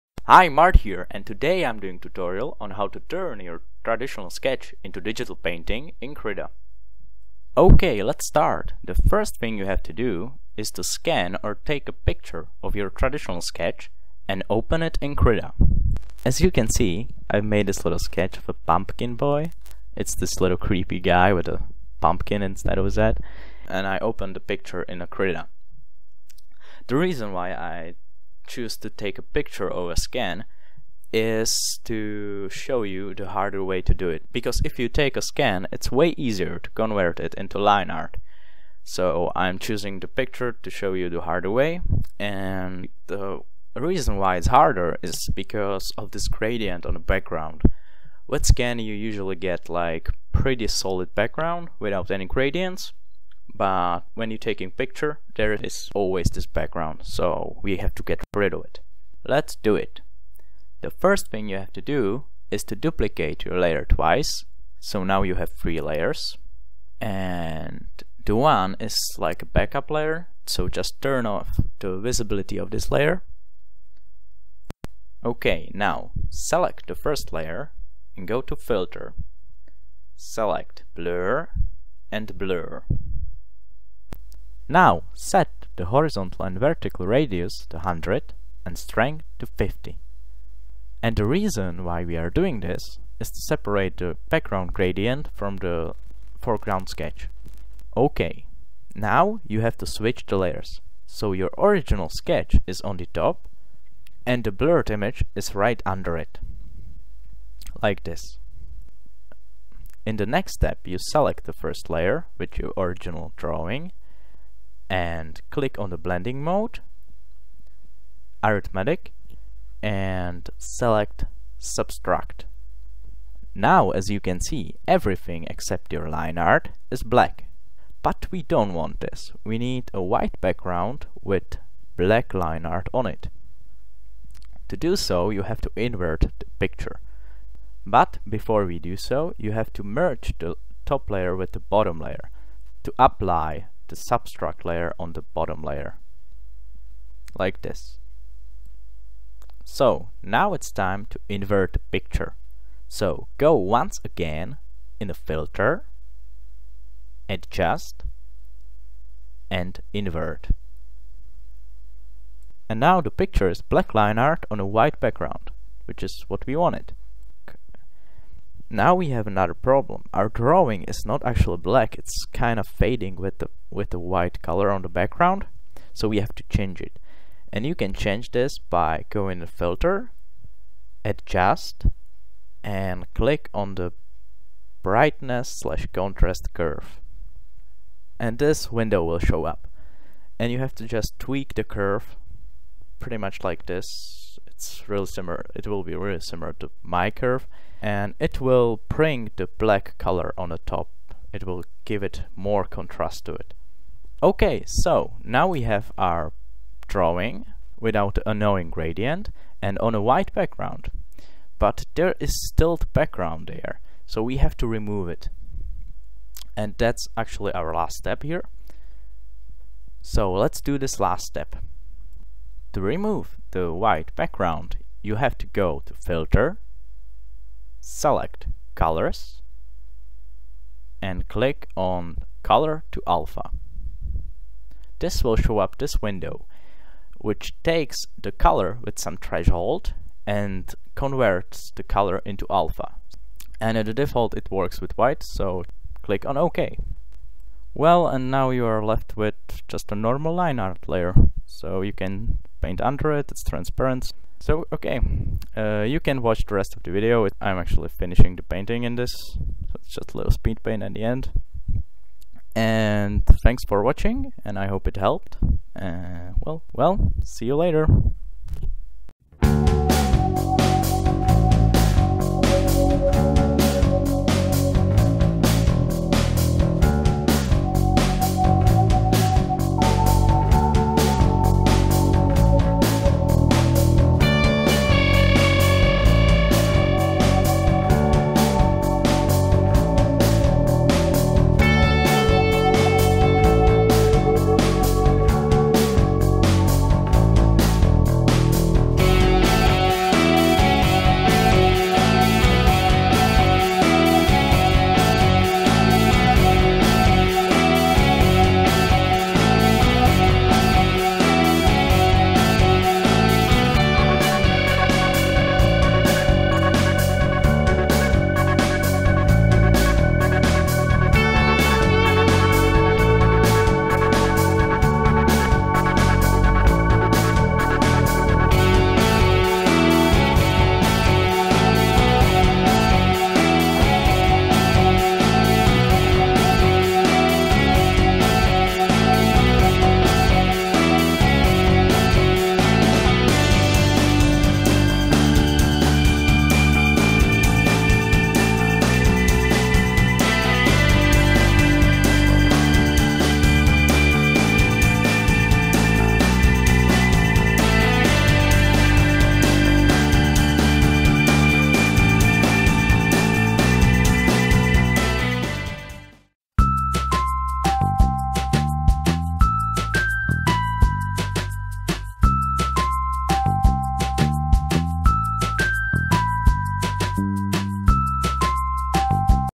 How to Turn Pencil Sketch in to Digital Painting in Krita 3
Hi Mart here,
Today I'm doing tutorial on how to turn your sketch in to digital painting In Krita, and also the halloween's here so I did little speedpaint for the spooktober ;)

btw. I'm so sorry about the sound quality, my mic sucks :(

Tools:
Krita 3.0.1
Audacity
Wacom Intuos Pro M
Blender
SimpleScreenRecorder